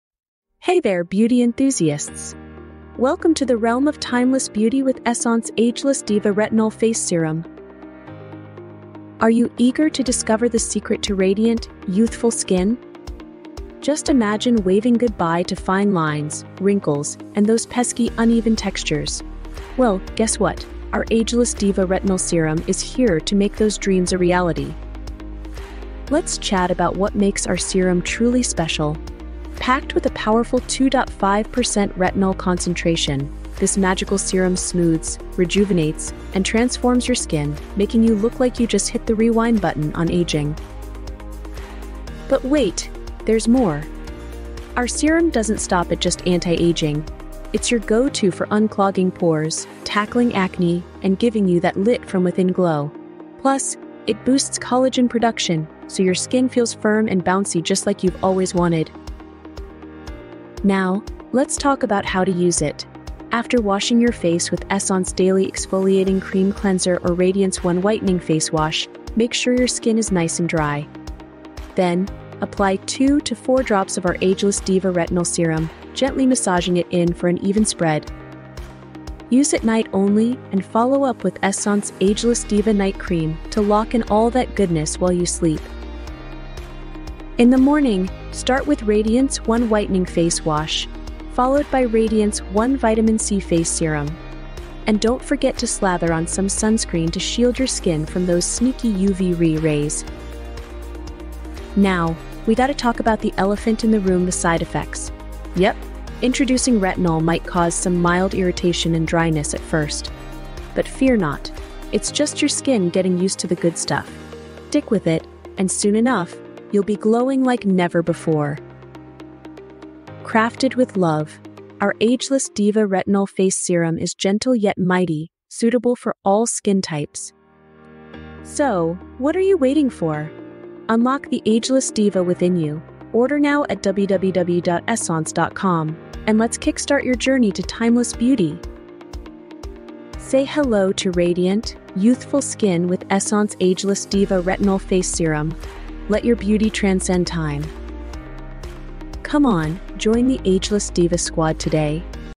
Essaunce Ageless Diva Retinol Face Serum
Unveil the Ageless Diva in You

Say hello to radiant, youthful skin with our powerhouse Ageless Diva Retinol Face Serum! 🌹✨

Key Benefits:
1. 2.5% Retinol Powerhouse:Supercharge your skincare routine with the potent magic of 2.5% retinol, known for its remarkable anti-aging properties. 🌟
2. Combat Wrinkles: Bid farewell to fine lines and wrinkles as our serum works tirelessly to smooth and rejuvenate your skin. 💆‍♀️
3. Unclog Pores: Get that fresh, clear complexion you've been dreaming of as our serum effectively unclogs pores, leaving your skin feeling refreshed and renewed. 🚿
4. Acne Targeting: Tackle pesky acne with the targeted action of our Ageless Diva serum, promoting a blemish-free, glowing complexion. 👊
5. Boost Collagen: Rejuvenate your skin's elasticity and firmness by boosting collagen production, giving you that coveted youthful bounce. 🌈
6. Line Smoothing: Experience the magic as our serum smoothes away those worry lines, leaving you with a flawless canvas. 🎨
7. Hydration Infusion: Dive into a sea of hydration with every application, as our serum quenches your skin's thirst for moisture. 💧

✨ How to Use:
Apply a few drops onto cleansed skin before your moisturizer. Use in your evening routine for optimal results. ✨

🌿 Gentle Formula, Powerful Results: Our Ageless Diva Retinol Face Serum is crafted with care, ensuring a gentle yet effective formula suitable for all skin types. 🌿

Your journey to timeless beauty starts here. ✨💖